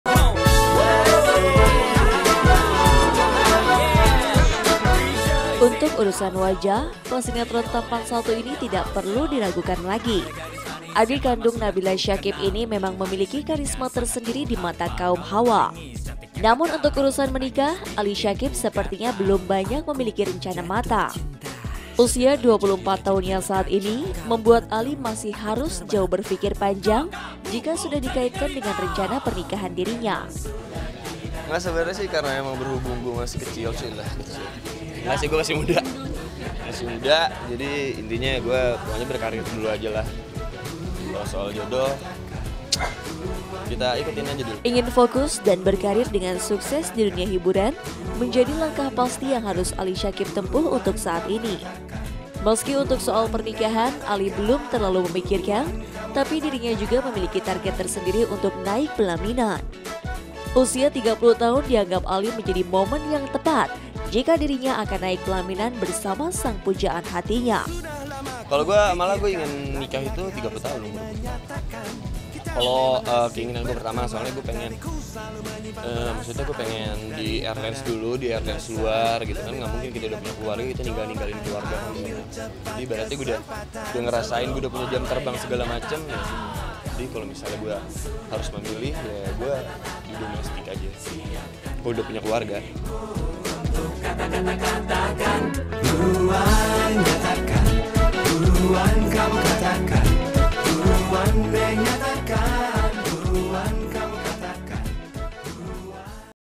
Usia 30, Ali Syakieb Wajib Nikah?
Sukses dengan kariernya di dunia hiburan sepertinya sudah cukup bagi perjalanan seorang Ali Syakieb. Kini adik kandung Nabila Syakieb itu hanya tinggal memikirkan masa depannya. Bahkan Ali sudah memiliki target menikah di usia 30 tahun. Benarkah seperti itu? Siapakah calon pendamping Ali Syakieb? Yuk intip beritanya di sini!